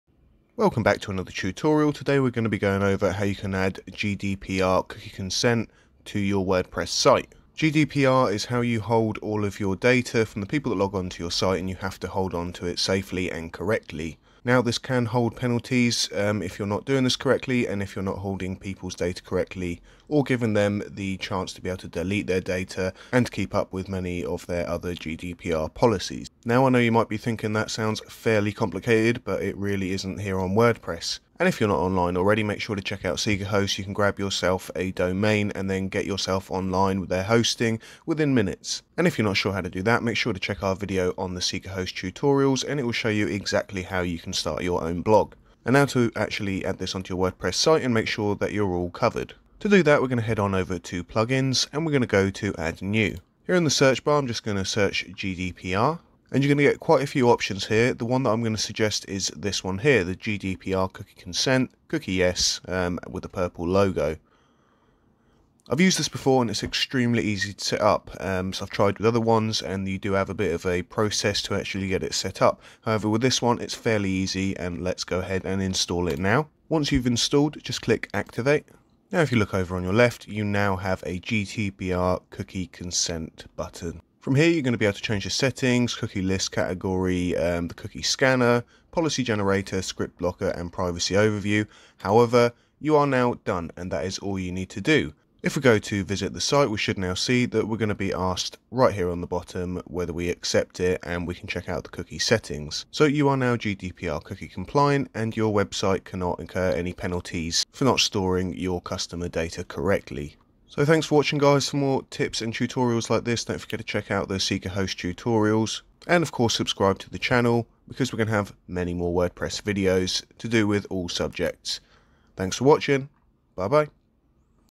How to Add GDPR Cookie Consent on WordPress | SeekaHost
Follow our quick and easy tutorial to install a plugin and make sure your site is abiding by the GDPR law.

Part of our series on How to Create a WordPress Blog/Site